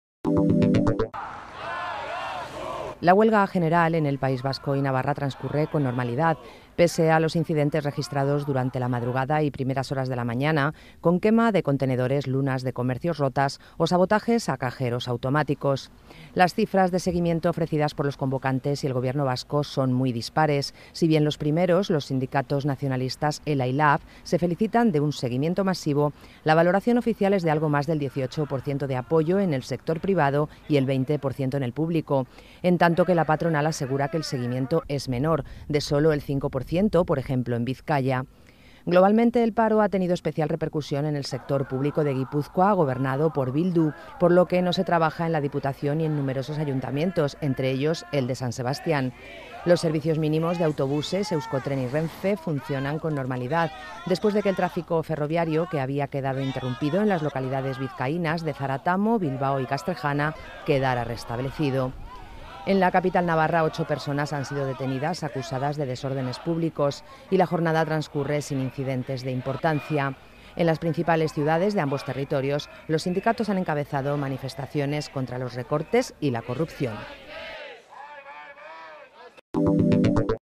Valoración dispar de la huelga en el País Vasco y Navarra
Vitoria, 30 may (EFE).-

El seguimiento de la huelga en el sector privado en el País Vasco es de un 18,7 % en el sector privado y de un 20 % en el ámbito público, según el Gobierno Vasco.

El seguimiento de la huelga en Álava en el ámbito privado se ha cifrado en 11,5 %, en Bizkaia en un 10,8 % y en Gipuzkoa en un 43,1 %, por lo que la media se sitúa en un 18,7 %, cuando en la de 26 de septiembre fue del 20 %.

En la Administración General Vasca, el seguimiento del paro de hoy se ha cifrado en un 22,31 %, frente a un 29,84 % en la de septiembre. En la Diputación de Álava se ha situado en un 15,2 % y en la de Bizkaia en un 14,4 %, mientras que la de Gipuzkoa, gobernada por EH Bildu que secunda el paro, hará públicos sus datos mañana.

Por su parte, la patronal vizcaína, CEBEK, ha asegurado que el seguimiento en este territorio de la huelga general convocada por los sindicatos nacionalistas no llega al 5 por ciento.

En una nota, CEBEK ha considerado "dispar" el seguimiento del paro en el comercio, con una mayor incidencia, del 25 %, en la Margen Izquierda y Gernika por la actuación de los piquetes y una media del 3 %.

En el metal el paro ha sido del 19 %, en la construcción del 12 %, en artes gráficas del 2,5 %, en servicios del 0,78 %, del 8 % en conserveras, del 4 % en los talleres y concesionarios y del 10 % en el transporte.